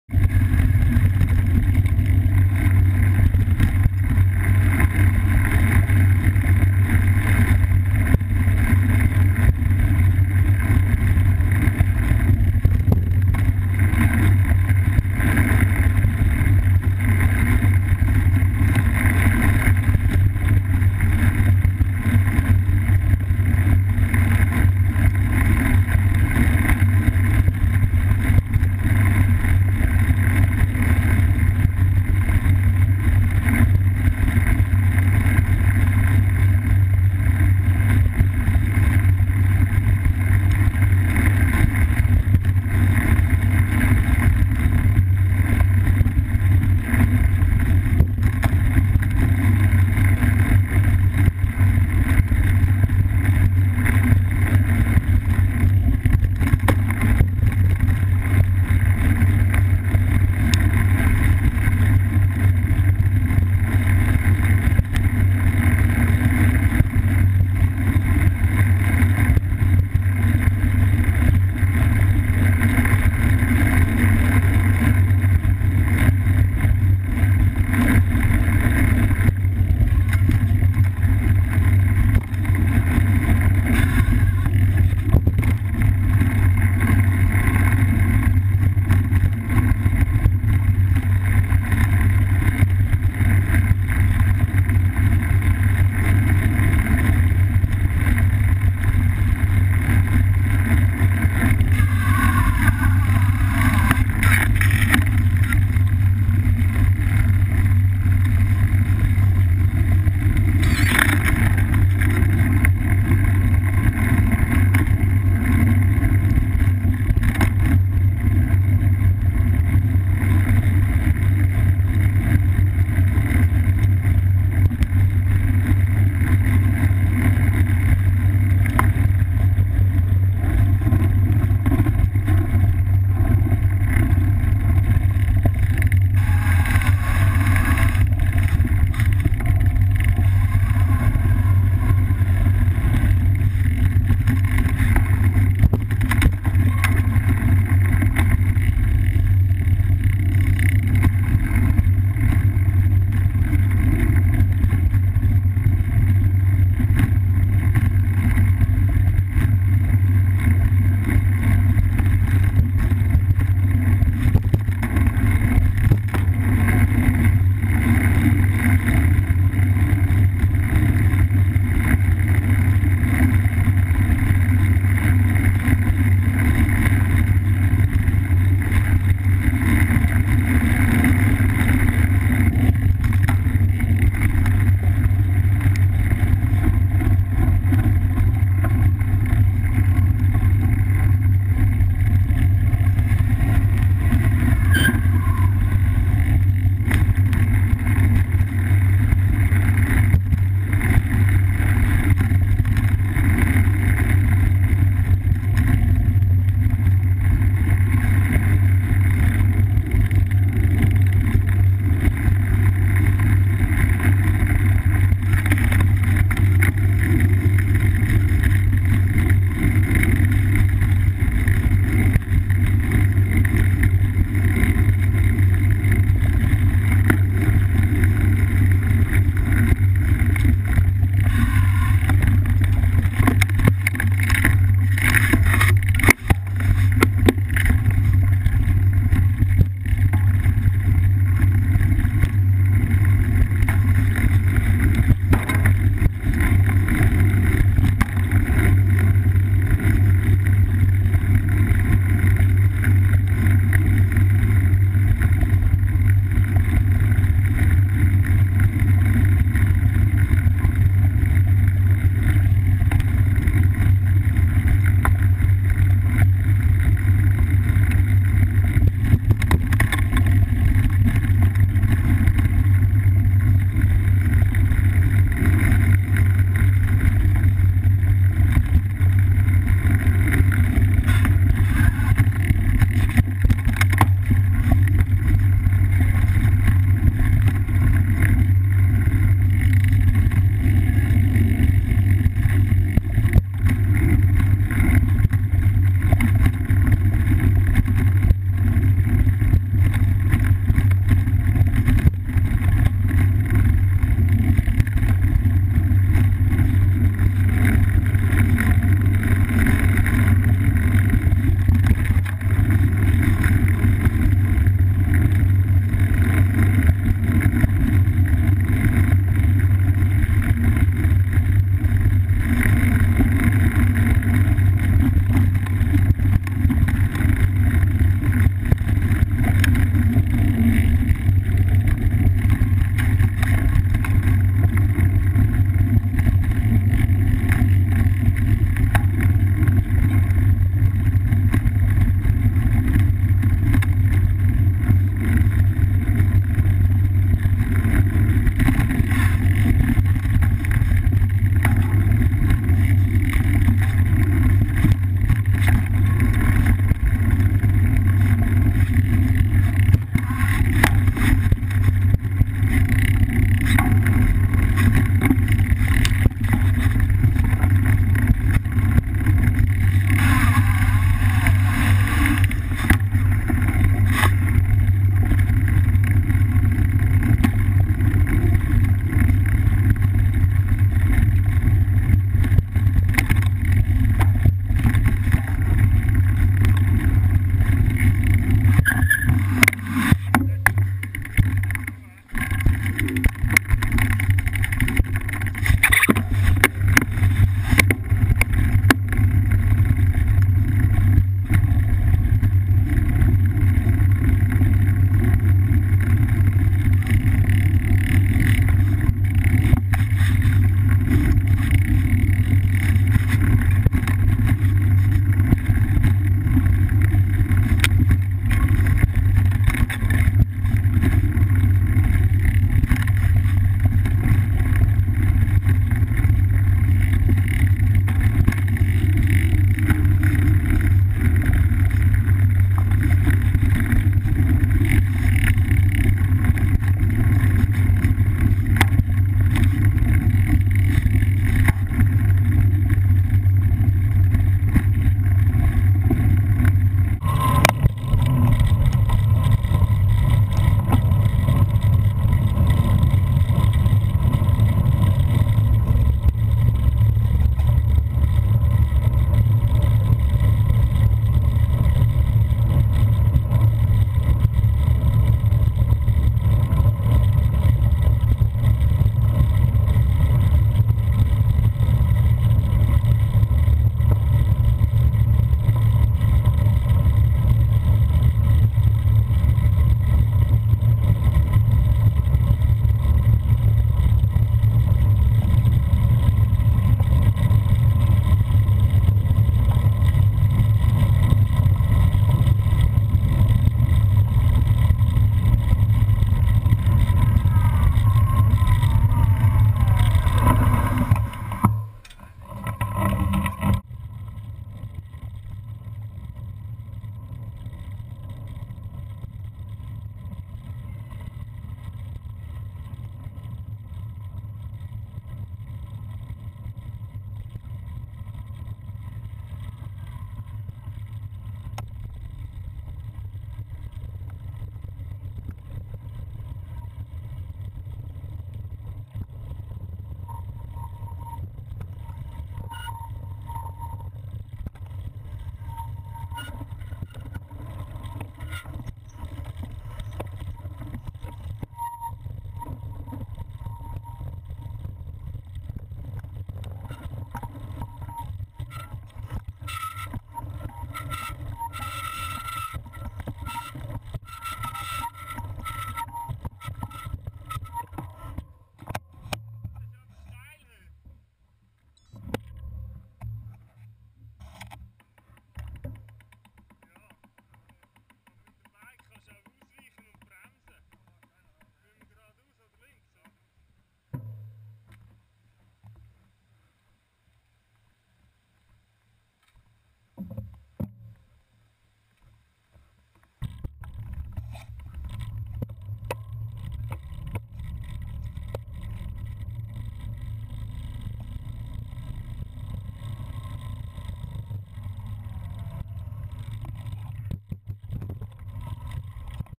CX singletrails
Franz Obrist and Christian Heule riding from Bassersdorf to Winterthur and back. 1h57riding time witch included about 1h of nice singletrails. sadly the movie needs to be less then 10min.
GoPro with k-edge mount on bar and saddlerails